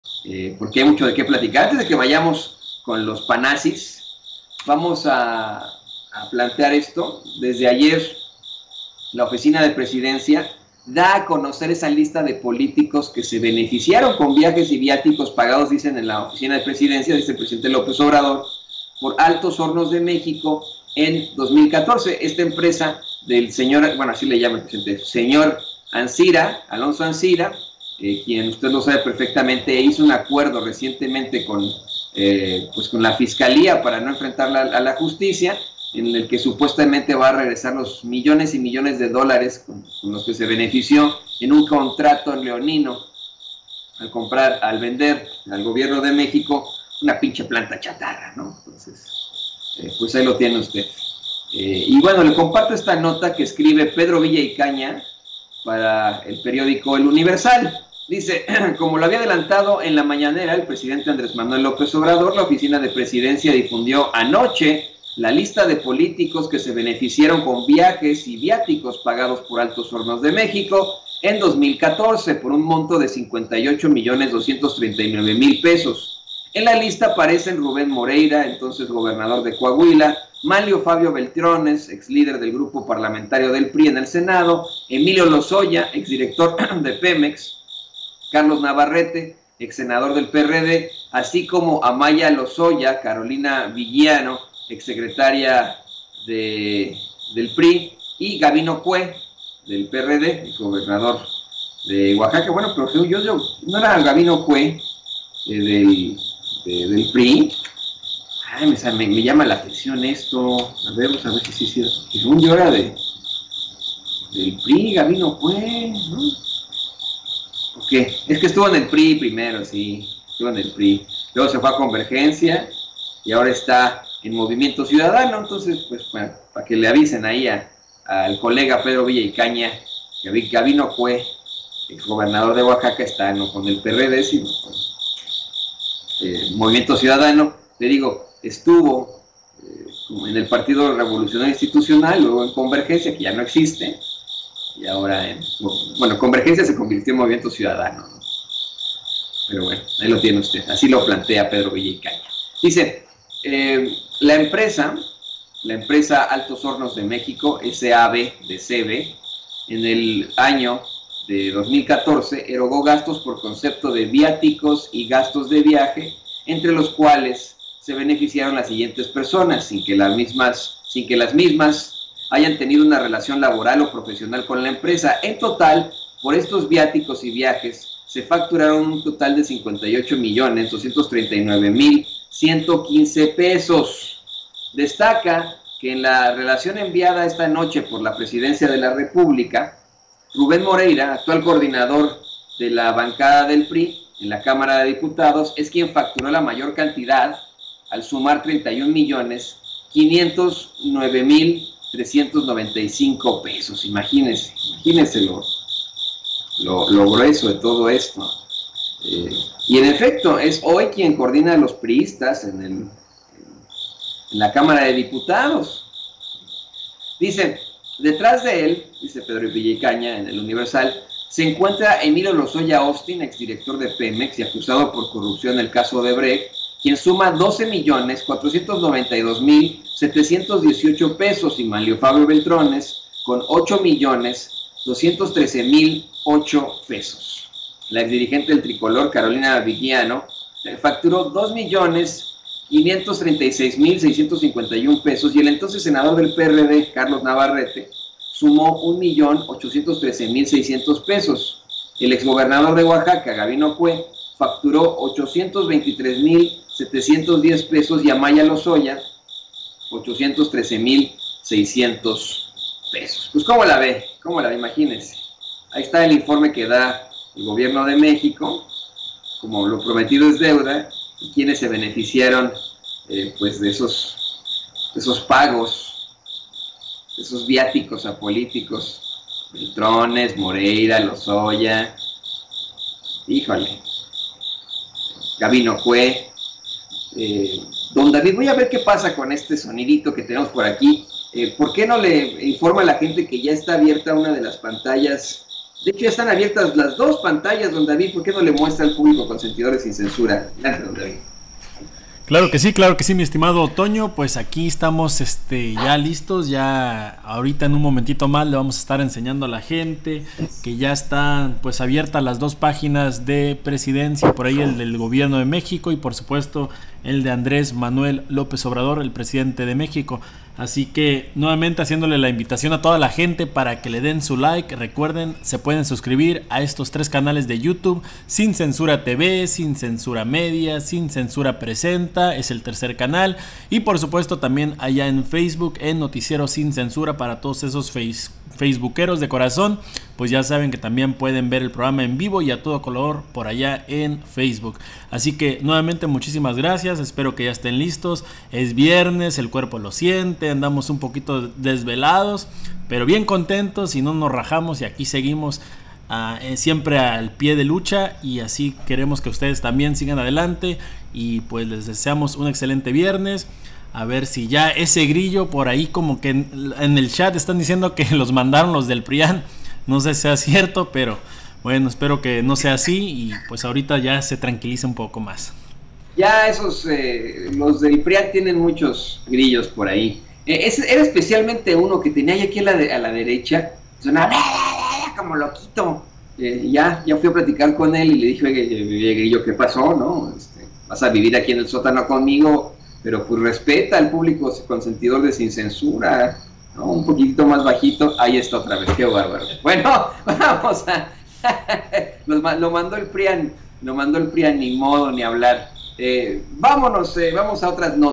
GOBIERNO DE AMLO REVELA PAGOS MILLONARIOS DE ANCIRA EN VIÁTICOS A POLÍTICOS
Como lo prometió el presidente López Obrador, su gobierno reveló a los políticos que se beneficiaron con 58 millones de pesos en viajes y viáticos de Altos Hornos de AlonsoAncira en 2014, entre ellos LozoyaAustin, Beltrones y por supuesto RubénMoreira.